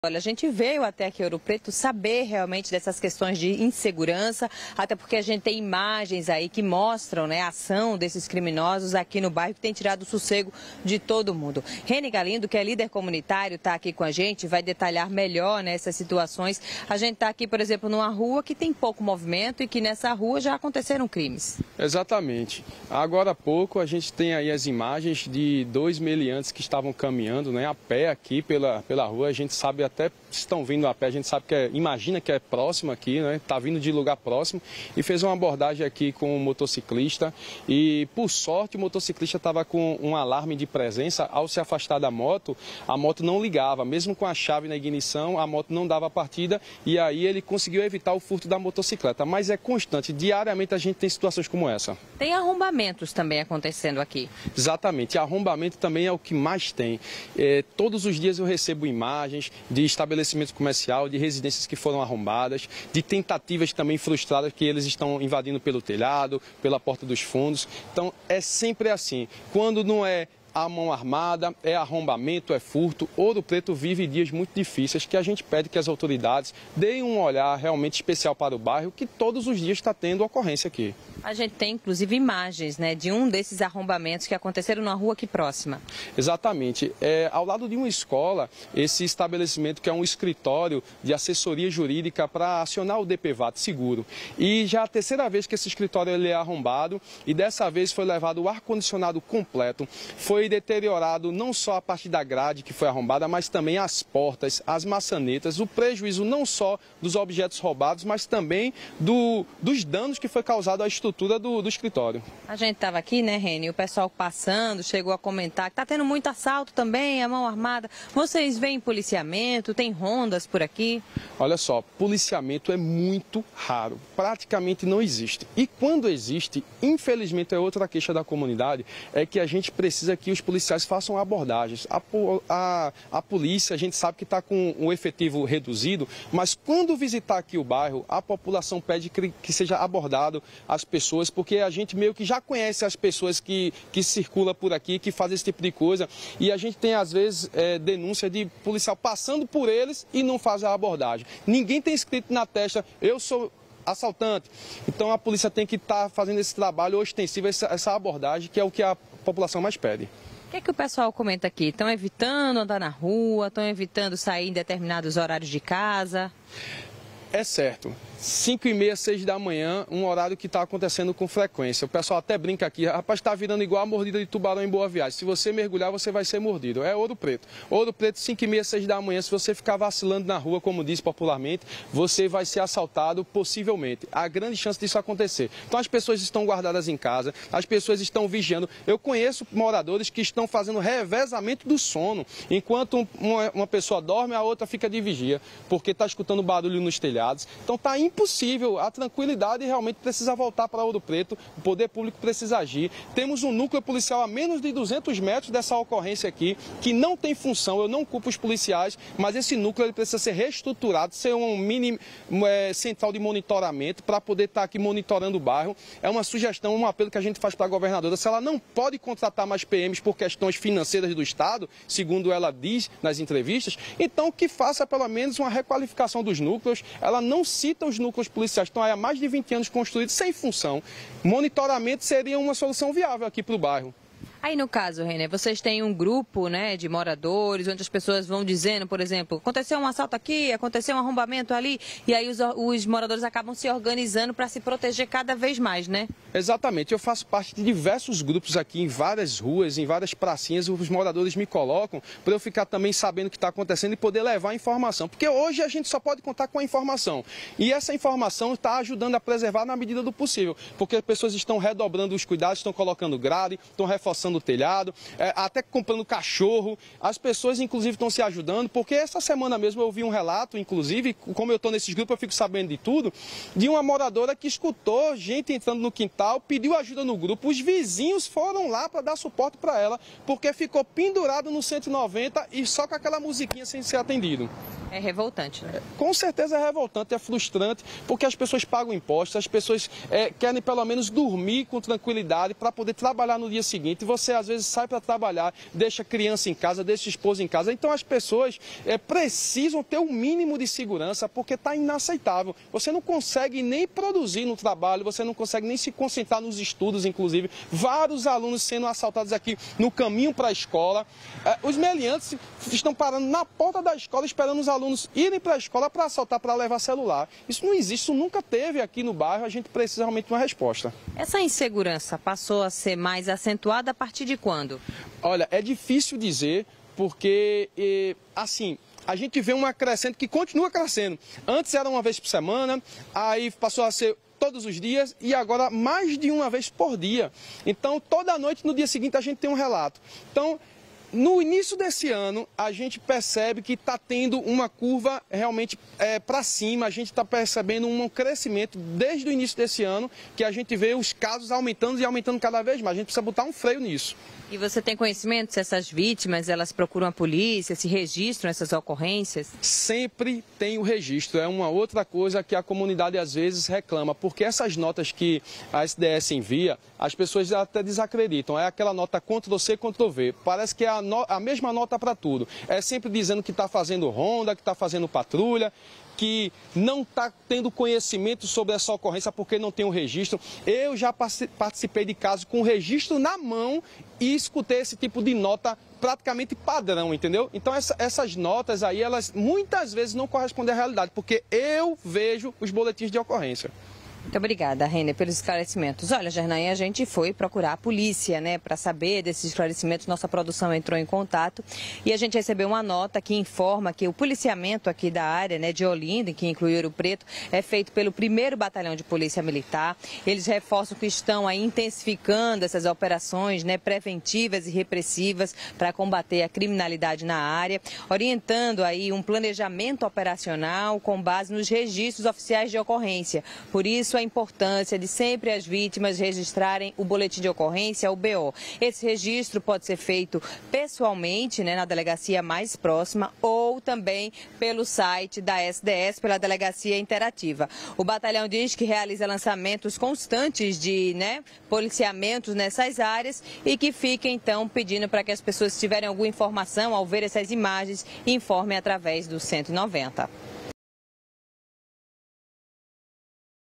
Moradores do bairro de Ouro Preto, em Olinda, estão assustados com a onda de assaltos na região
Moradores do bairro de Ouro Preto, em Olinda, têm vivenciado momentos de preocupação devido à crescente onda de assaltos na região. Relatos dos moradores revelam que roubos e tentativas de roubos nas ruas de Ouro Preto têm se tornado rotina diária.